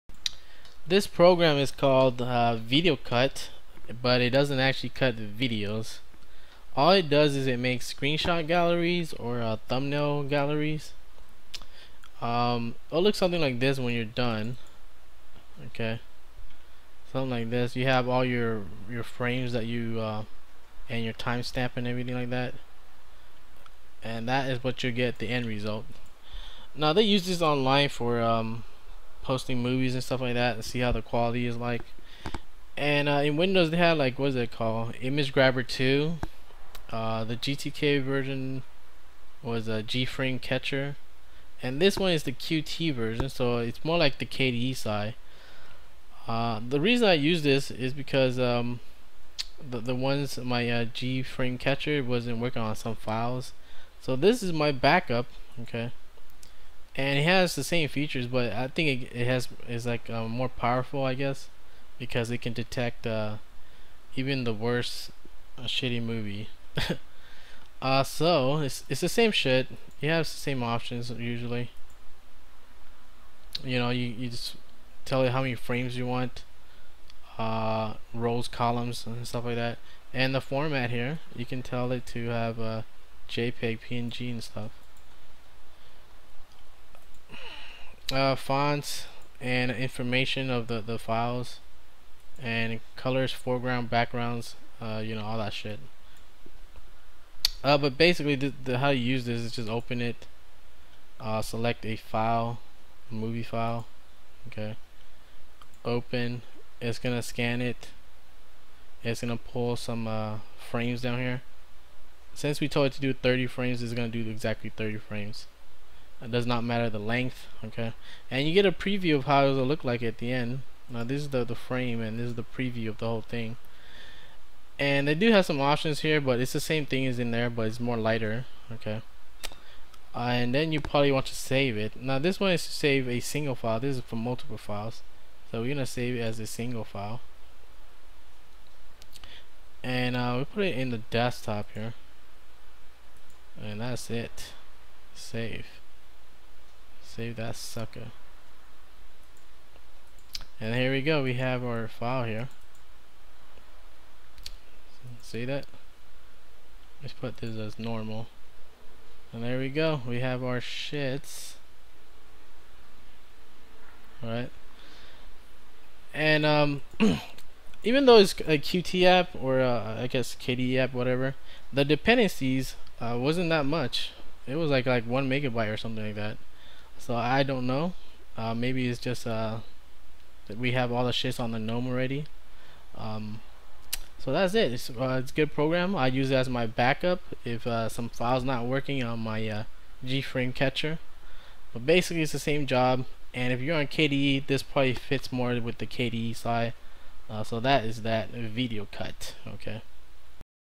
Screenshot Gallery - VideoCut - Linux Mint 7
$ sudo apt-get install videocut

VideoCut is an open source desktop application specialized for
 creating compositions of screenshots from video files.
Additional you can also export your whole composition in one
image file with additional audio and video header informations.